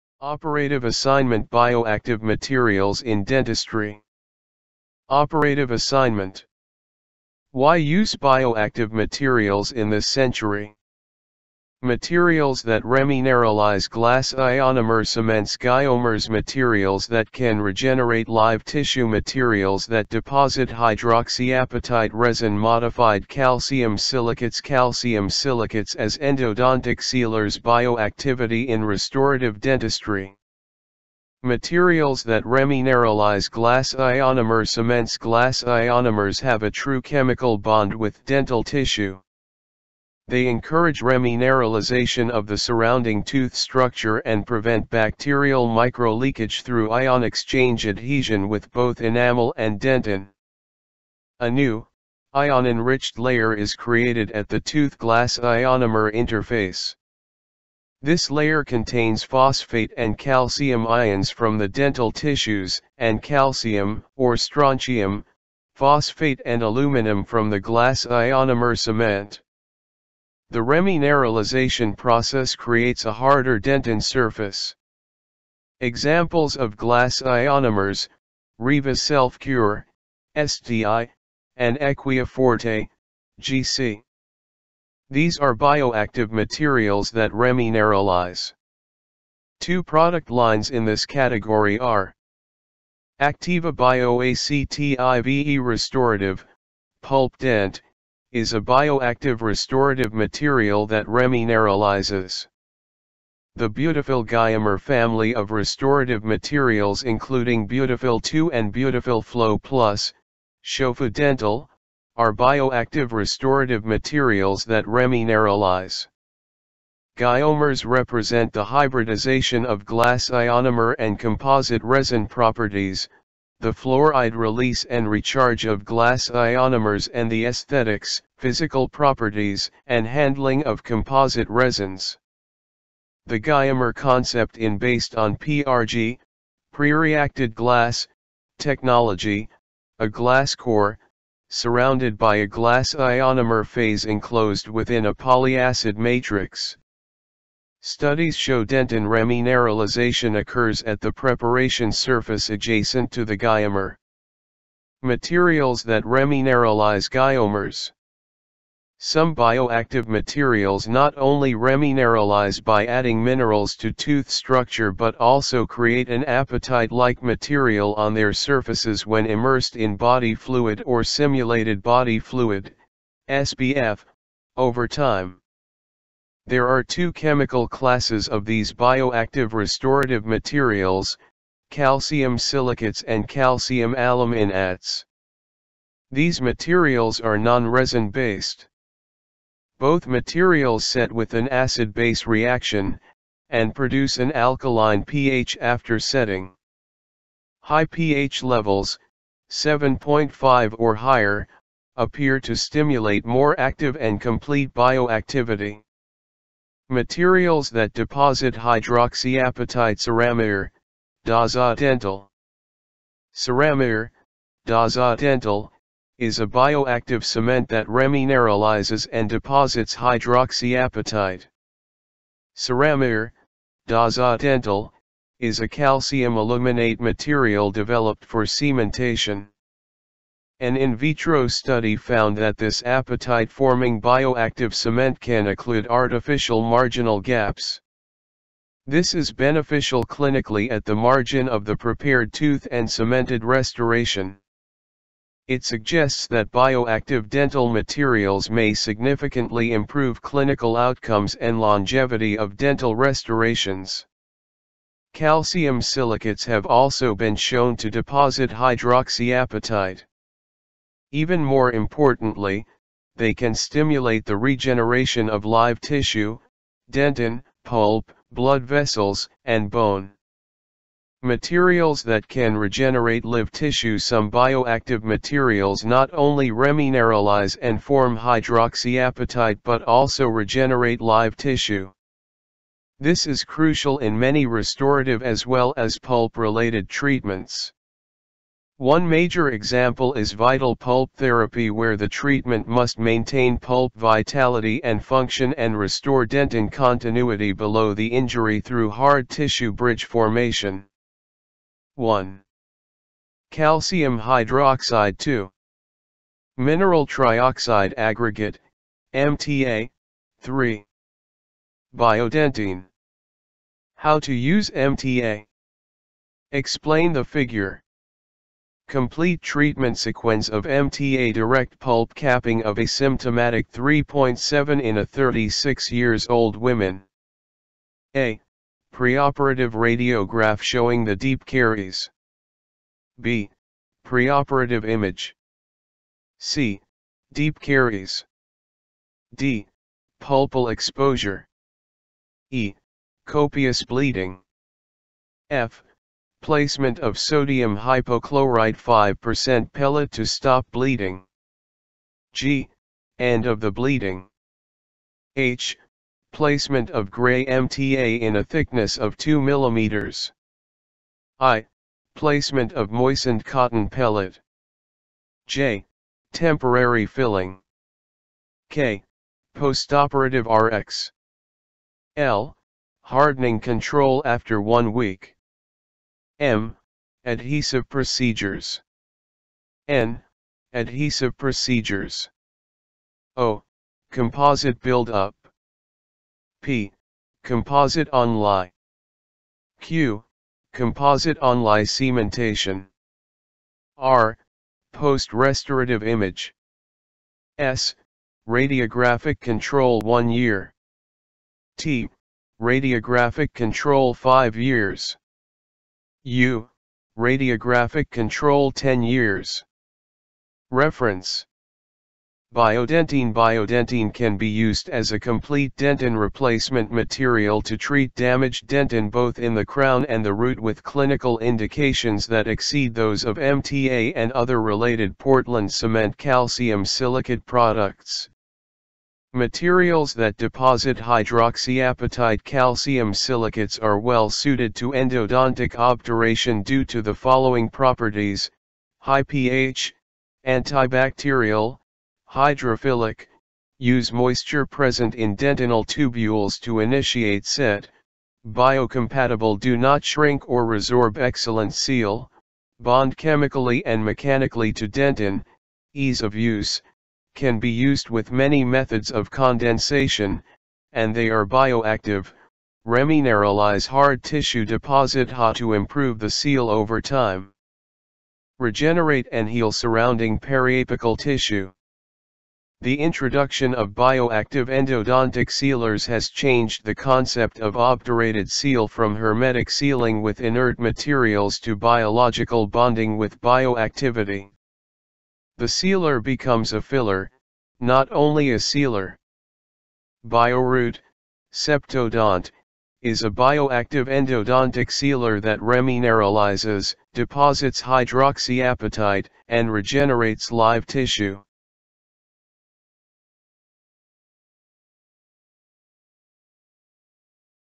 Operative assignment Bioactive materials in dentistry
Operative Assignment.
 Why Use Bioactive materials in this century?
● Materials that remineralize Glass Ionomer Cements Giomers ● Materials that can regenerate live tissue ● Materials that deposit hydroxyapatite Resin Modified Calcium Silicates Calcium Silicates as Endodontic Sealers Bioactivity In Restorative Dentistry
● Materials that remineralize Glass Ionomer Cements ● Glass ionomers have a true chemical bond with dental tissue. ● They encourage remineralization of the surrounding tooth structure and prevent bacterial microleakage through ion-exchange adhesion with both enamel and dentin. ● A new, ion-enriched layer is created at the tooth-glass ionomer interface. This layer contains phosphate and calcium ions from the dental tissues, and calcium (or strontium), phosphate and aluminum from the glass ionomer cement. remineralization process creates a harder dentin surface.
Examples of glassionomers, Riva Self Cure (SDI) and Equia Forte (GC). These are bioactive materials that remineralize.
Two product lines in this category are:
 Activa BioACTIVE Restorative (Pulpdent) is a bioactive restorative material that remineralizes. Beautifil giomer family of restorative materials including Beautifil II and Beautifil Flow Plus (Shofu Dental) are bioactive restorative materials remineralize.
 Giomers represent the hybridization of glass ionomer and composite resin properties: fluoride release and recharge of glass ionomers and the esthetics, physical properties, and handling of composite resins. The giomer concept in based on PRG (Pre-Reacted Glass) technology: a glass core, surrounded by a glass ionomer phase enclosed within a polyacid matrix. Studies show dentin remineralization preparation surface adjacent to the giomer. ● Materials that remineralize Giomers
 Some bioactive materials not only remineralize by adding minerals to tooth structure but also create an apatite-like material on their surfaces when immersed in body fluid or simulated body fluid (SBF) over time. There are two chemical classes of these bioactive restorative materials: calcium silicates and calcium aluminates. These materials are non-resin based. Both materials set with an acid-base reaction, and produce an alkaline pH after setting. High pH levels (7.5 or higher) appear to stimulate more active and complete bioactivity. ● Materials that deposit hydroxyapatite Ceramir (Doxa Dental)
 Ceramir (Doxa Dental) is a bioactive cement that remineralizes and deposits hydroxyapatite. Ceramir (Doxa Dental) is a calcium aluminate material developed for cementation. An in vitro study found that this apatite-forming bioactive cement can occlude artificial marginal gaps. This is beneficial clinically at the margin of the prepared tooth and cemented restoration. It suggests that bioactive dental materials may significantly improve clinical outcomes and longevity of dental restorations. Calcium silicates have also been shown to deposit hydroxyapatite. Even more importantly, stimulate the regeneration of live tissue – dentin, pulp, blood vessels and bone.
 ● MATERIALS THAT CAN REGENERATE LIVE TISSUE Some bioactive materials not only remineralize and form hydroxyapatite but also regenerate live tissue.crucial in many restorative , pulp related treatments. One major example vital pulp therapy Treatment must maintain pulp vitality and function and restore dentin continuity injury through hard tissue bridge formation. 1. Calcium hydroxide 2. Mineral trioxide aggregate (MTA) 3. Biodentine
 How to use MTA
 Explain the Figure. Complete treatment sequenze of MTA direct pulp capping asymptomatic 3.7 in a 36 years old women. (A) Preoperative radiograph showing the deep caries. (B) Preoperative image. (C) Deep caries. (D) Pulpal exposure. (E) Copious bleeding. (F) Placement of Sodium Hypochlorite 5% pellet to stop bleeding. (G) End of the bleeding. (H) Placement of grey MTA in a thickness of 2 mm. (I) Placement of moistened cotton pellet. (J) Temporary filling. (K) Postoperative Rx. (L) Hardening control after 1 week. (M) Adhesive procedures. (N) Adhesive procedures. (O) Composite build-up. (P) Composite Onlay. (Q) Composite onlay cementation. (R) Post restaurative image. (S) Radiographic control 1 year. (T) Radiographic control 5 years. (U) Radiographic control 10 years. 크라운 브리지 치아
 Biodentine Biodentine used complete dentin replacement material to treat damaged dentin both in crown root with clinical indications exceed MTA Portland cement calcium silicate products.